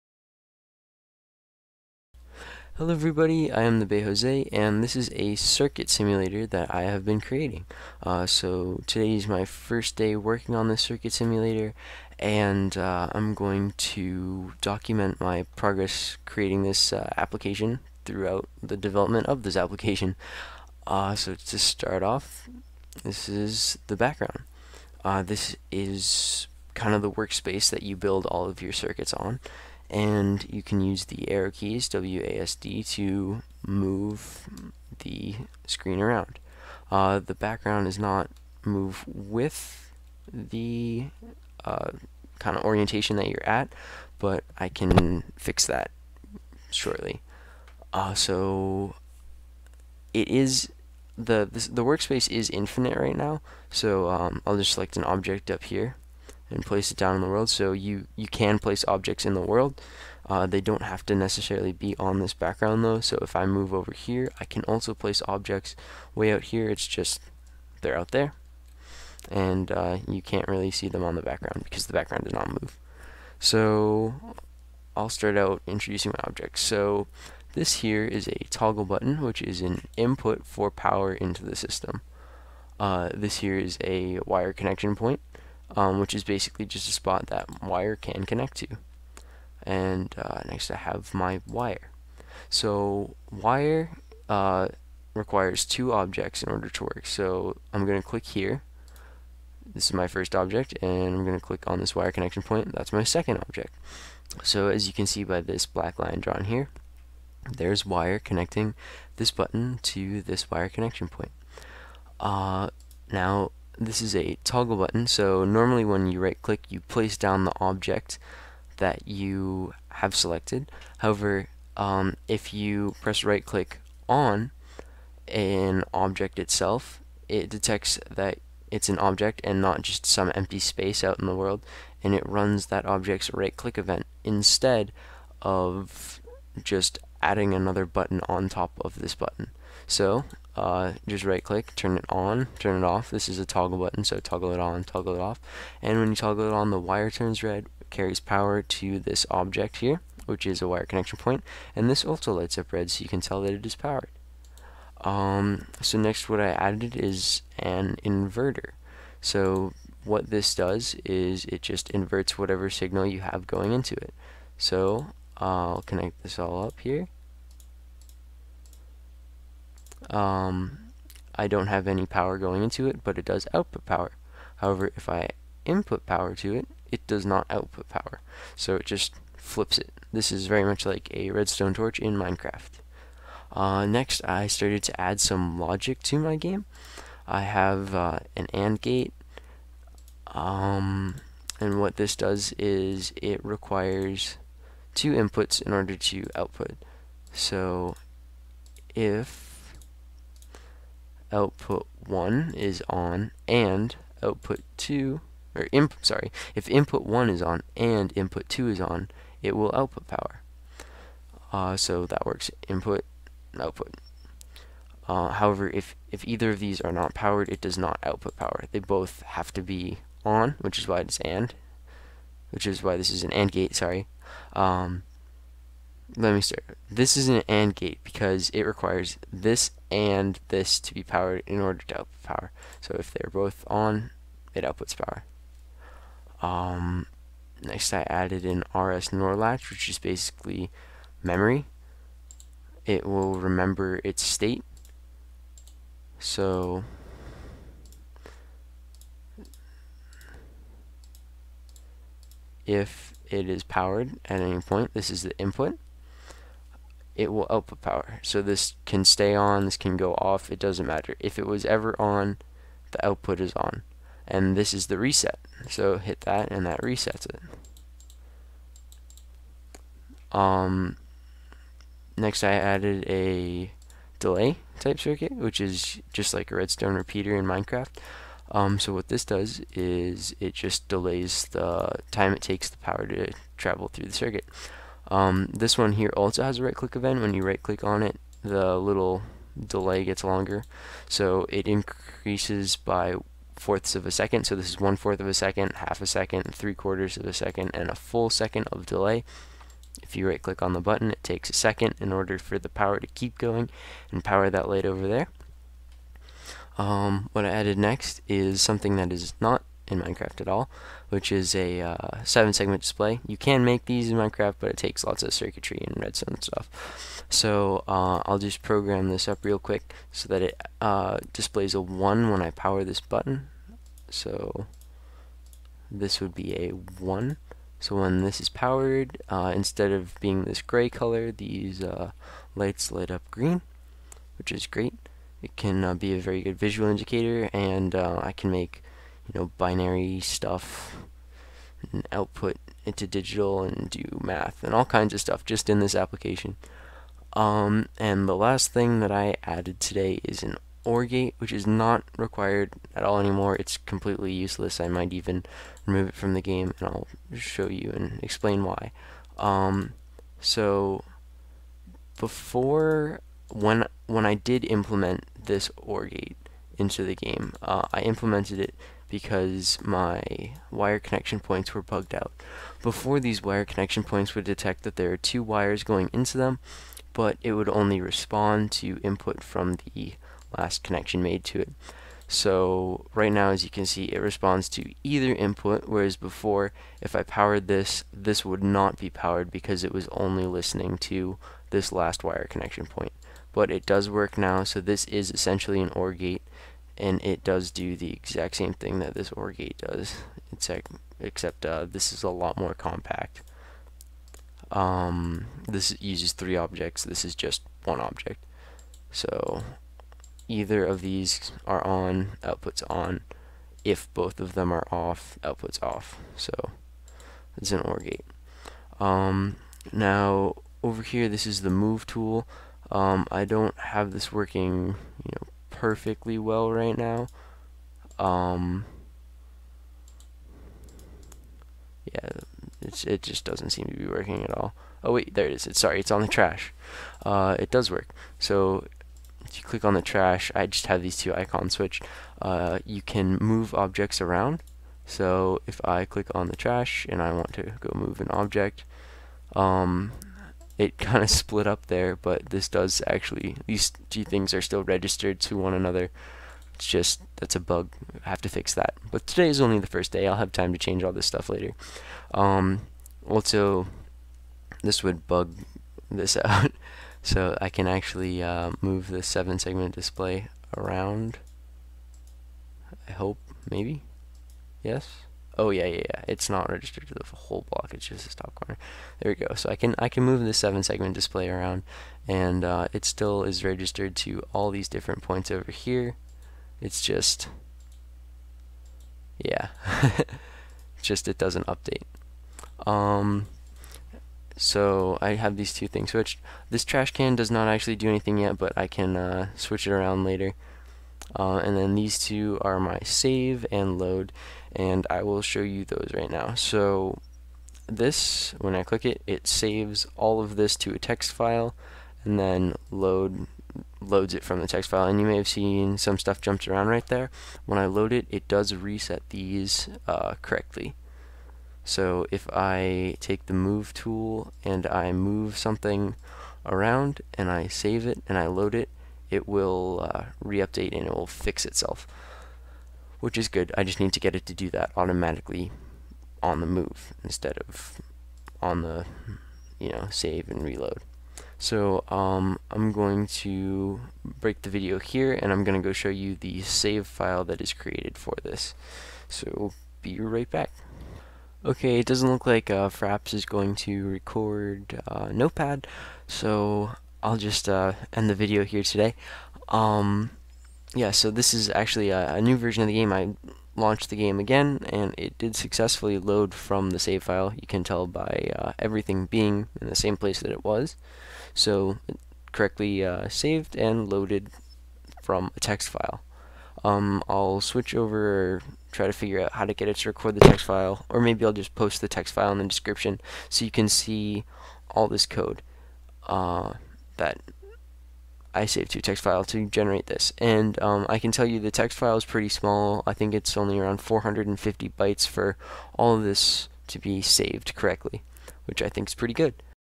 Circuit Simulator Dev :Friday (day 1)
this is an application i made today. this application allows you to create Circuit. this is day one of development so there are still many bugs and unimplemented features/.